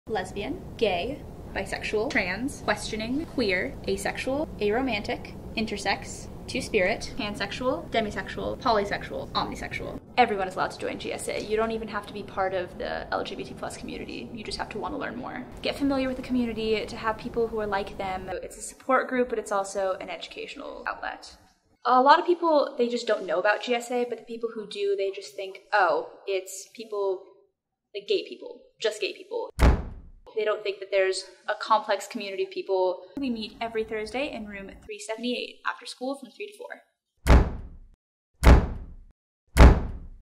KSMS GSA Promo
Alex creates a promo for our school's GSA organization